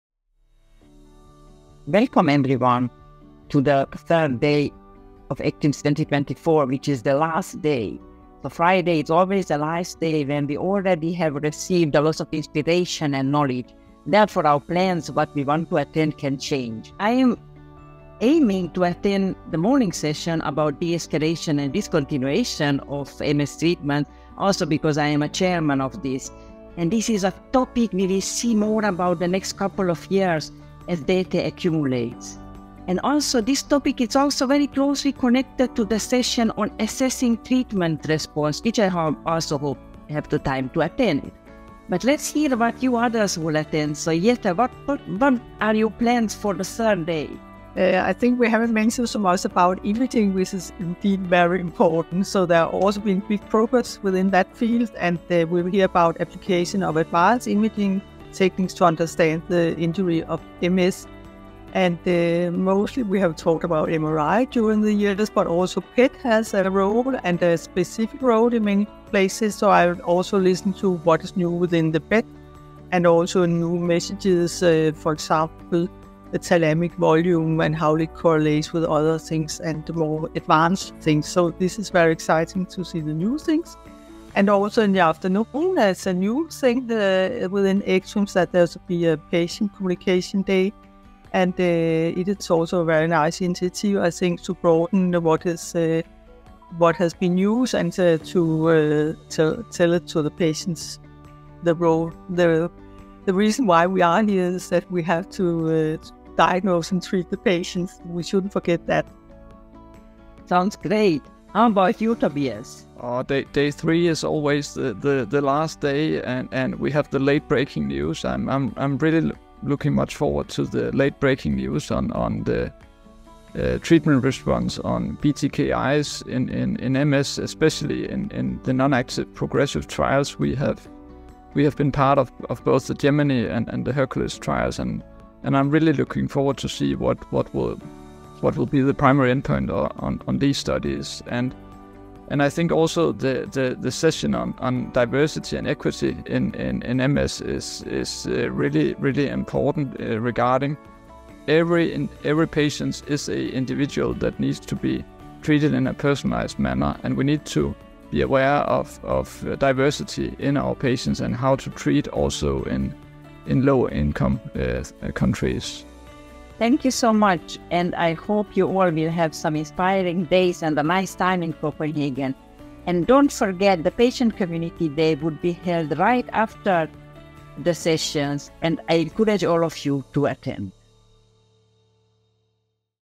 ECTRIMS 2024: Daily Highlights - Day 3
Welcome to the first day of the 40th Congress of the European Committee for Treatment and Research in Multiple Sclerosis. Watch MS expert Professor Melinda Magyari from the Danish Multiple Sclerosis Registry, alongside Professor Jette Frederiksen from the Danish Multiple Sclerosis Centre and Professor Tobias Sejbæk from the University of Southern Denmark, as they present today’s Daily Highlights.